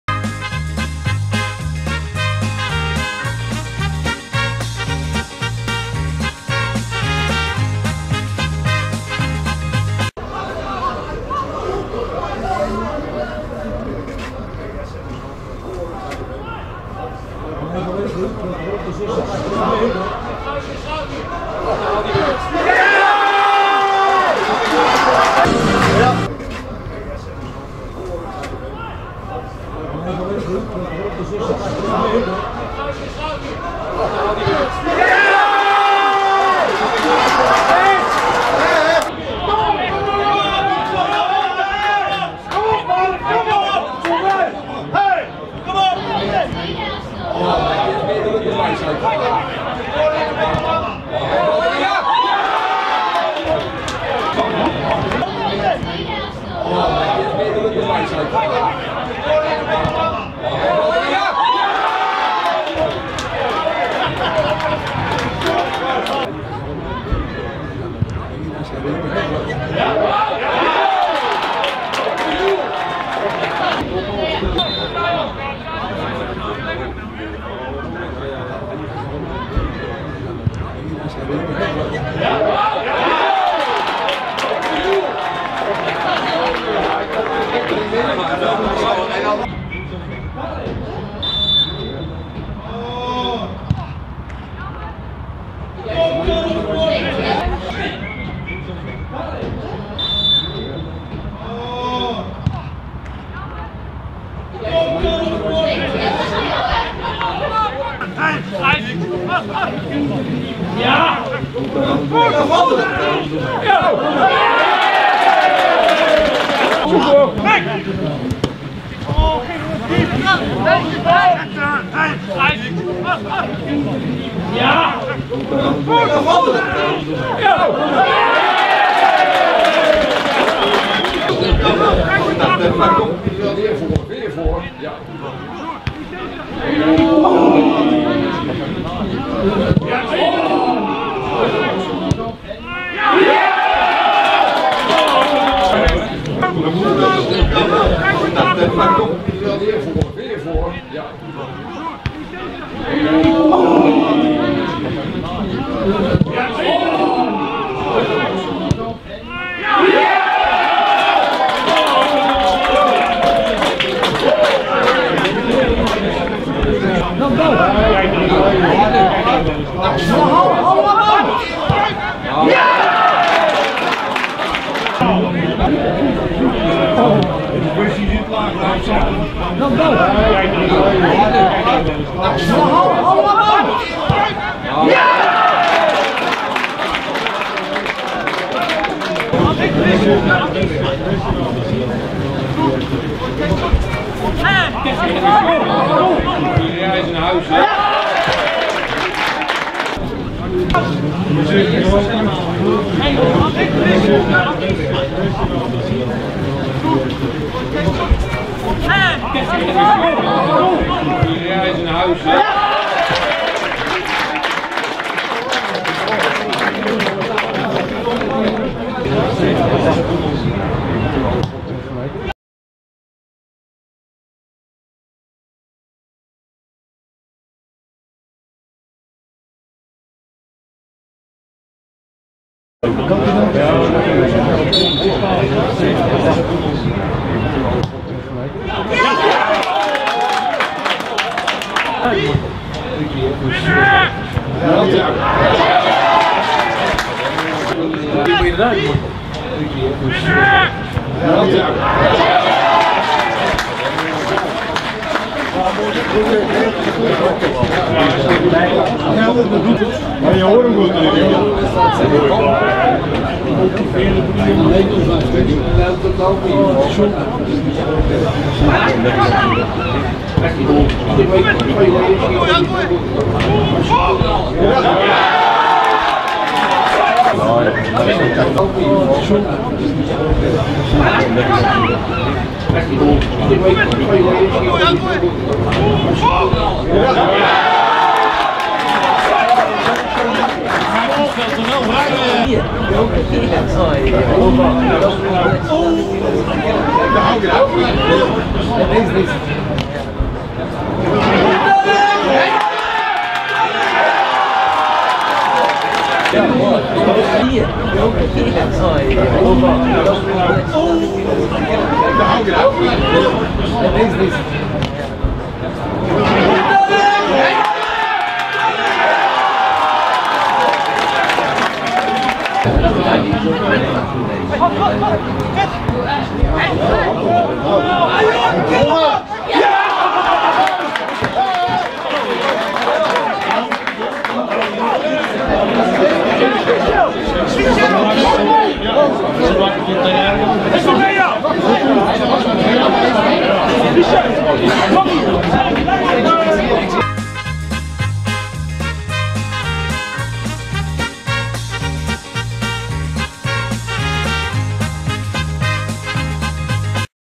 Deel 3 videosamenvatting doelpunten seizoen 2018-2019
Met Sliedrecht - GRC '14 1-0, Altena - NOAD '32 2-1,  LRC - Geinoord 4-0 en Schelluinen - SVW 3-2